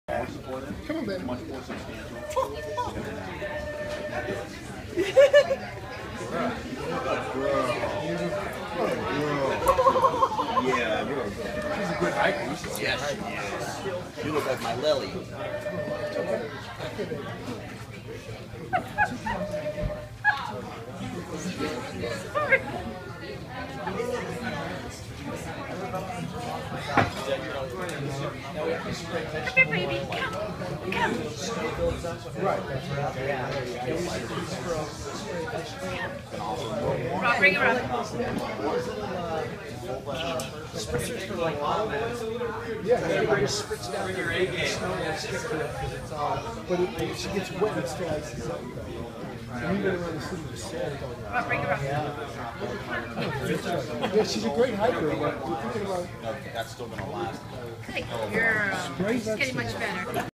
Dog walking in dog hiking shoes
Bertha not enjoying walking in the hiking shoes.